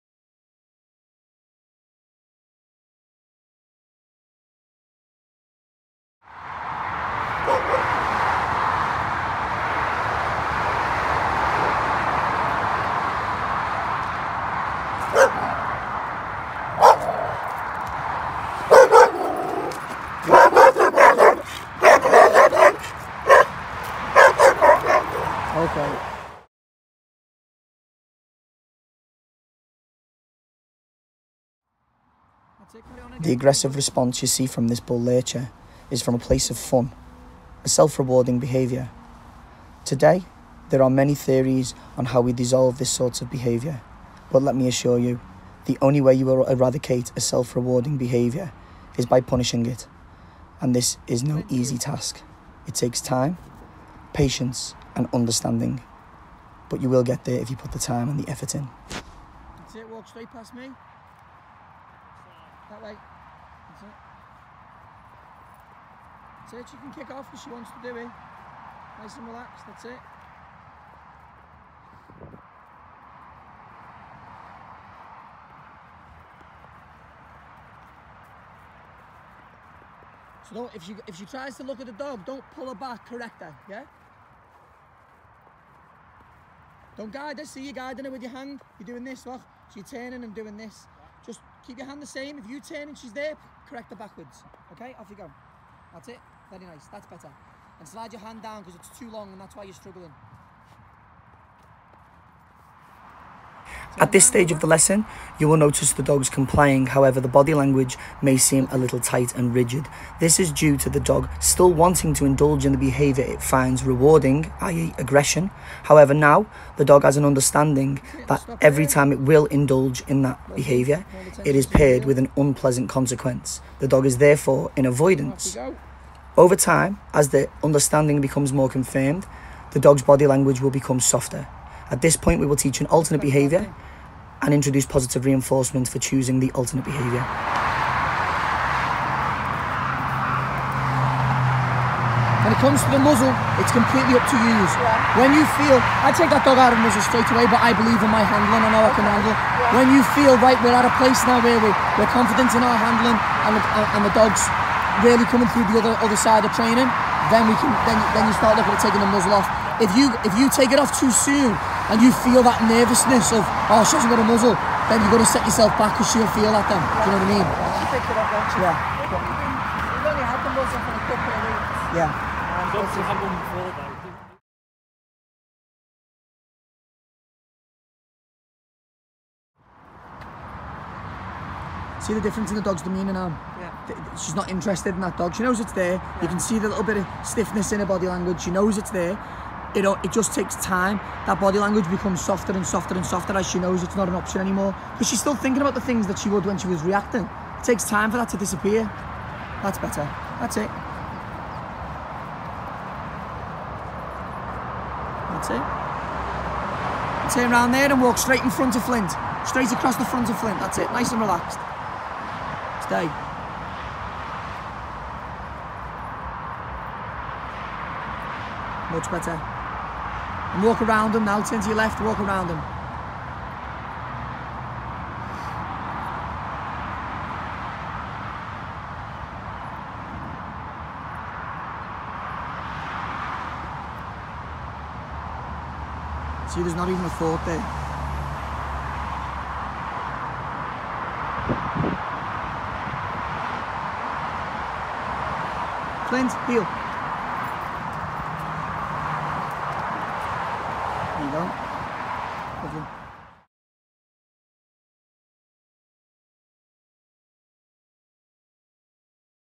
Dog reactive Bull-Lurcher
This Bull-Lurcher was adopted from a rescue centre by a caring family.  Yet again, this caring family were not informed of the dogs issues, putting them and other people’s dogs in harms way!  They sort both help and advice from ‘todays’ behaviourists (ticklers) and were told to always walk the dog in a muzzle and basically avoid everything that triggered the dogs aggression.  This is impossible!  They were also told that this dog was acting in such a way because she is scared.  That statement alone screams out how little these people know about dogs.  This dog was purpose bred to be a killing machine!! It’s doing what it’s genetics tell it to do.  So we went to work!